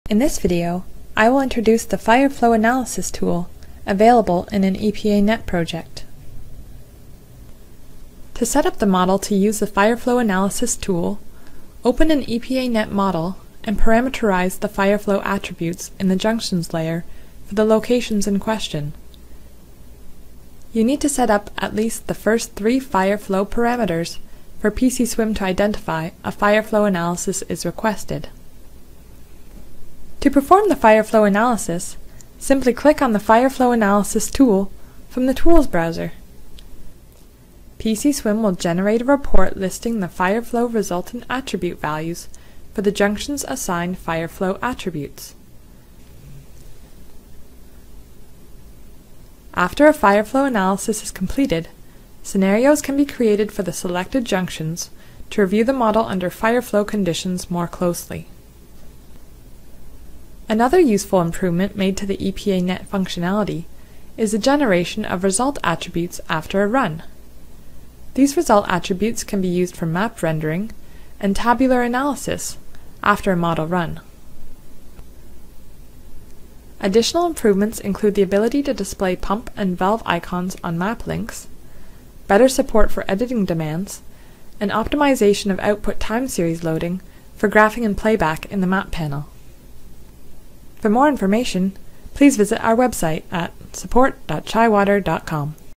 What's new in PCSWMM 7.2: Improved EPANET support
In this video, I will introduce the fire flow analysis tool, available in an EPANET project.